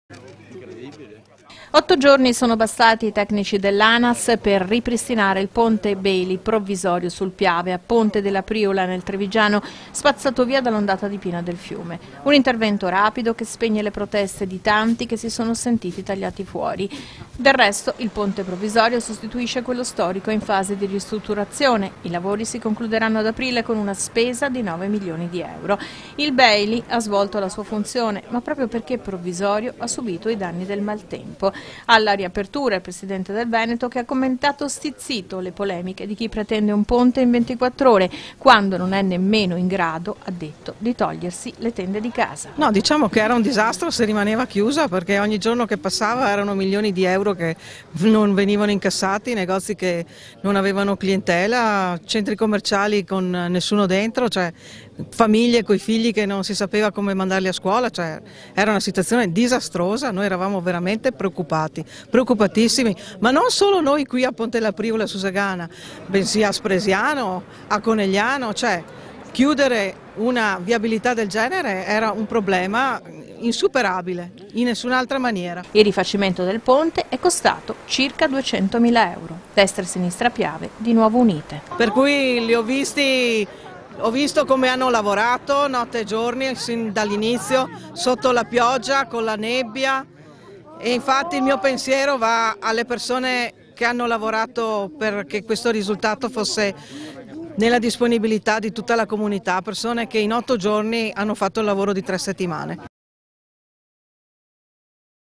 RIAPRE IL PONTE
Riaperto ad una settimana di distanza dalla piena del fiume, il Ponte sul Piave a Ponte della Priula. Un intervento in tempi rapidi. A fine aprile dovrebbero concludersi i lavori di ristrutturazione del Ponte storico